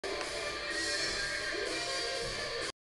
Hickok 534A v1.3
Tube Tester. Vintage, one of the BEST in the market. A classic piece. Hickok 534A. Ser# 0572